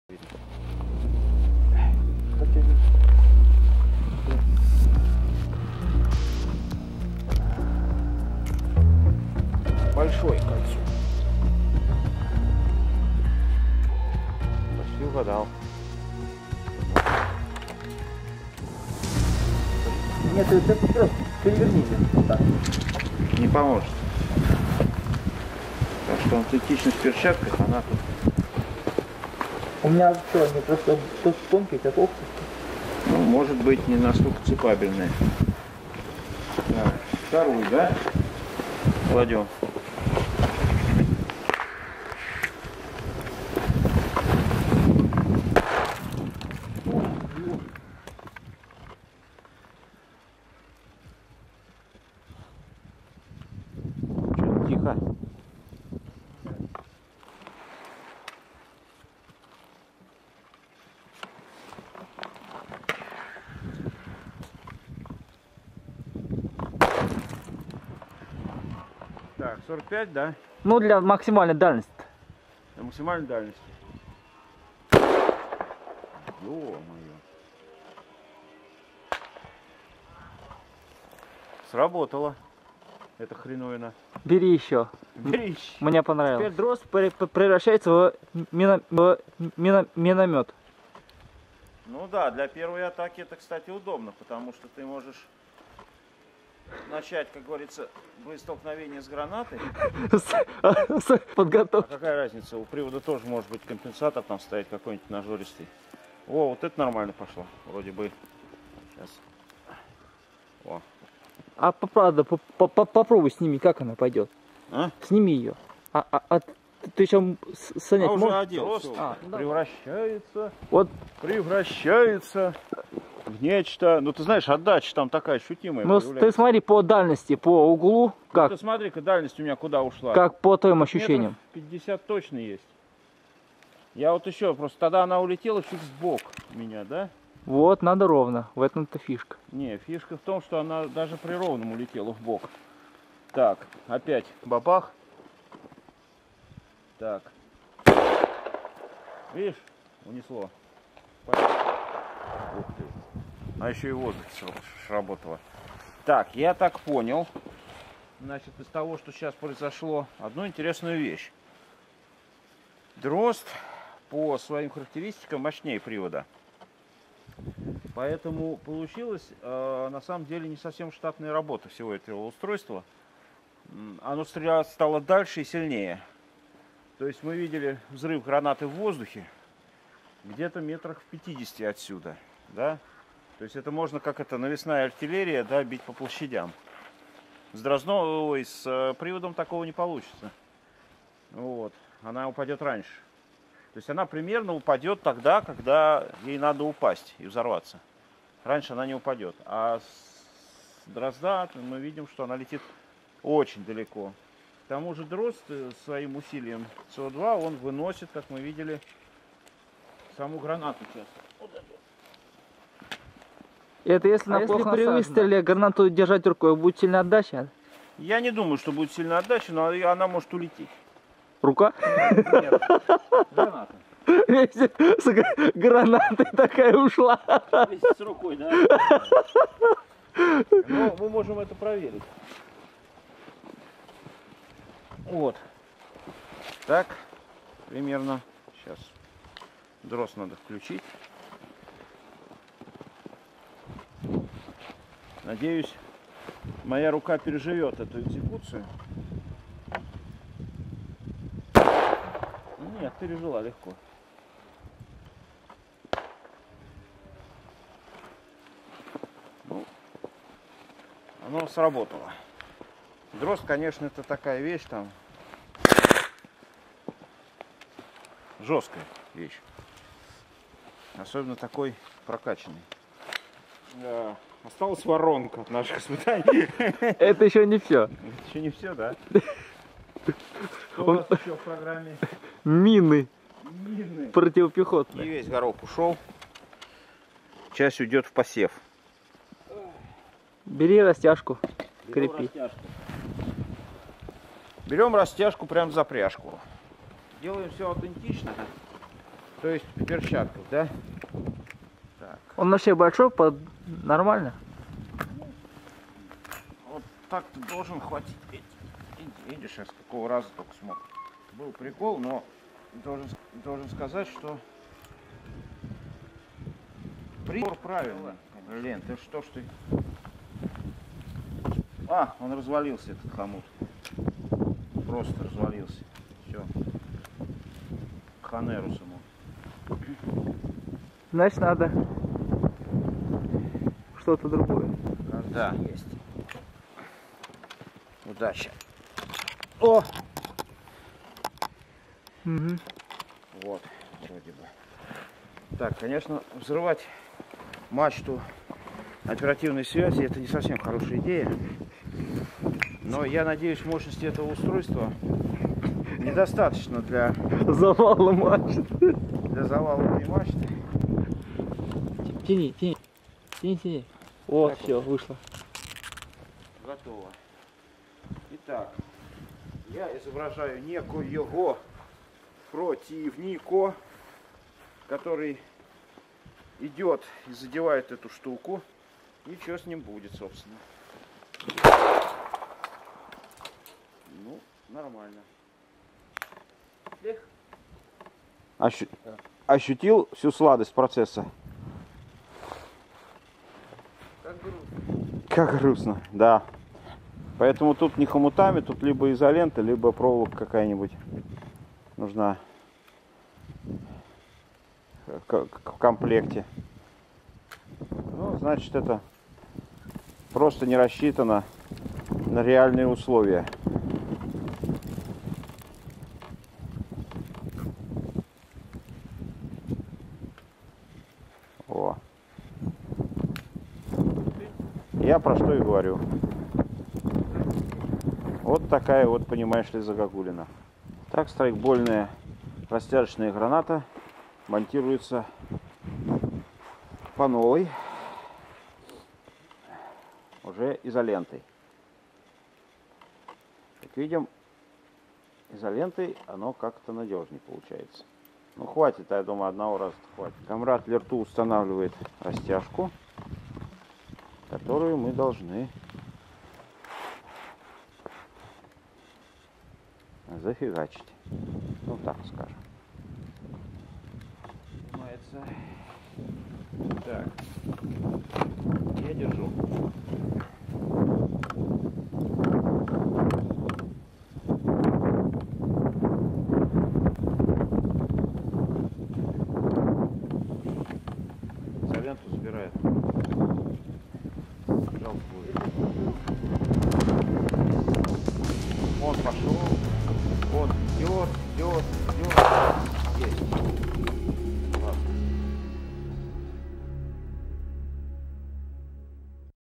РГС6, ММС-растяжка, СНГ-калибр - испытания страйкбольных гранат
Проверка работоспособности и эффекта, а так же полевых тонкостей страйкбольных гранат для использования в Лесном снайпинге. В частности, СНГ-Калибр запускался с МР-661 "Дрозд".
Испытатели: Lertu и Bolo